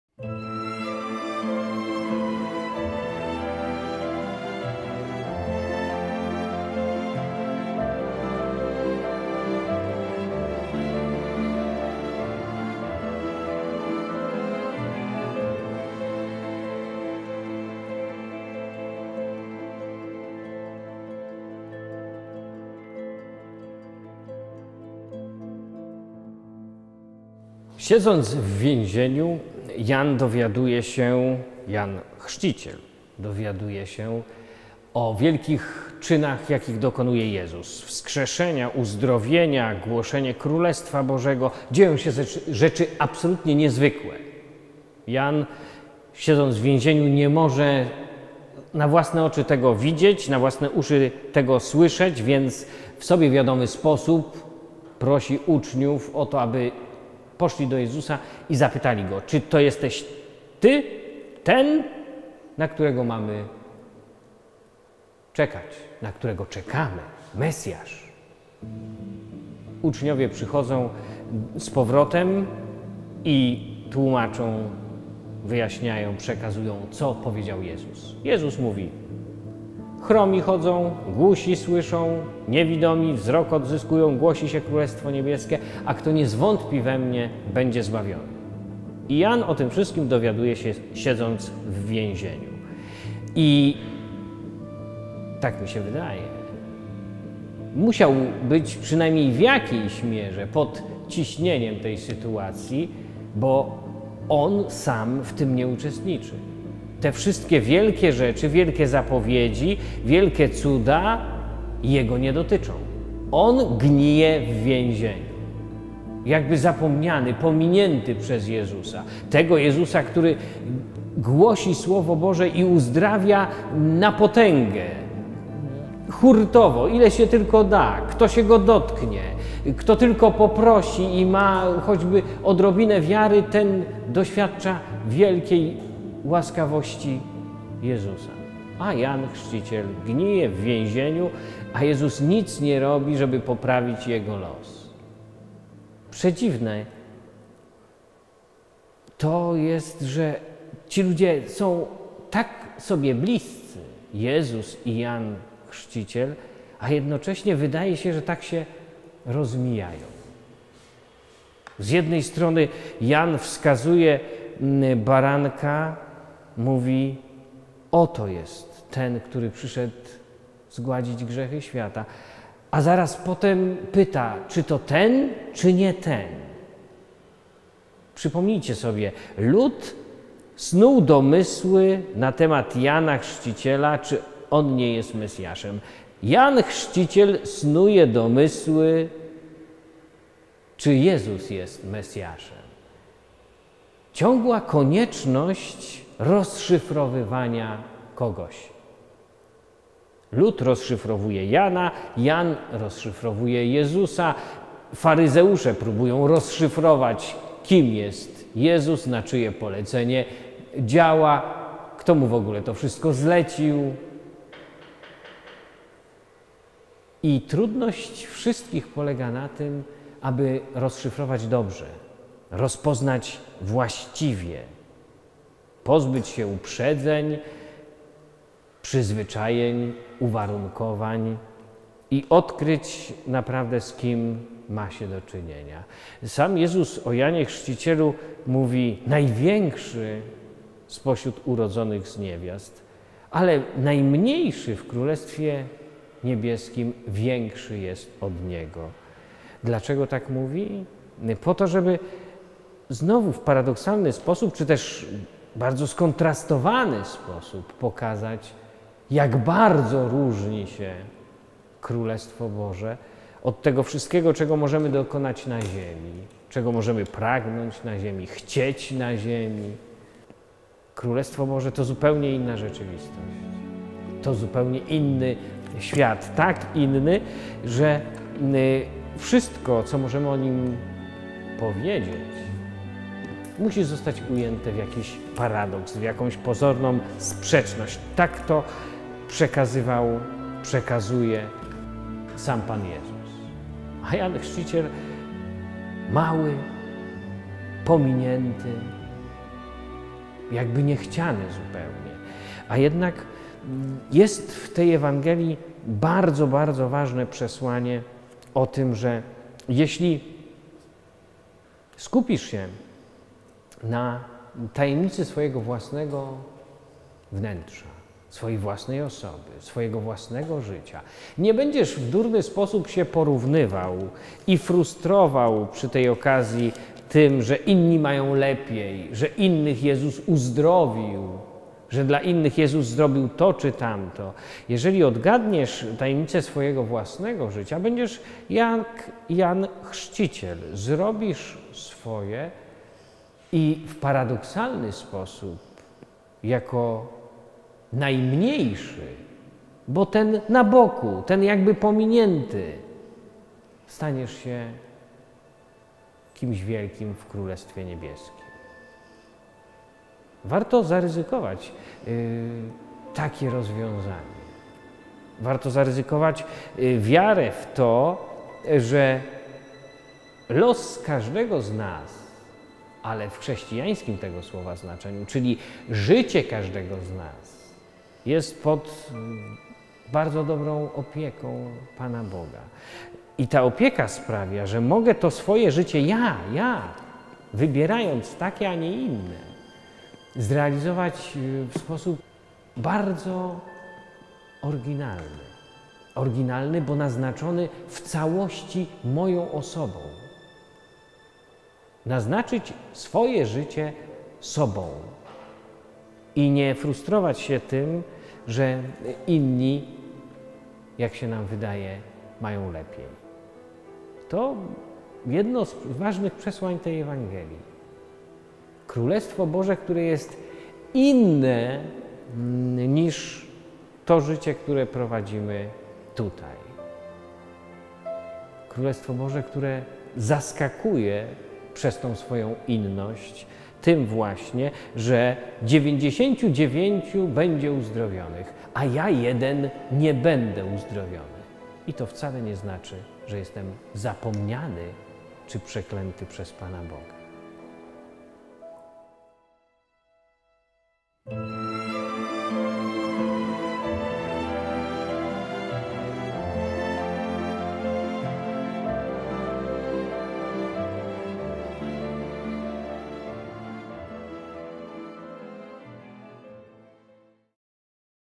Boskie Słowa - III niedziela Adwentu, 2013
Między narodzonymi z niewiast nie powstał większy od Jana Chrzciciela. Lecz najmniejszy w królestwie niebieskim większy jest niż on...

Jeśli skupisz się na tajemnicy swojego własnego wnętrza, nie będziesz w durny sposób się porównywał z innymi i przy tej okazji frustrował... Będziesz jak Jan Chrzciciel, zrobisz swoje i jako najmniejszy staniesz się kimś wielkim w Królestwie Niebieskim... Warto zaryzykować wiarę w to, że życie każdego z nas jest pod opieką Boga i to, że mogę swoje życie zrealizować w sposób bardzo oryginalny...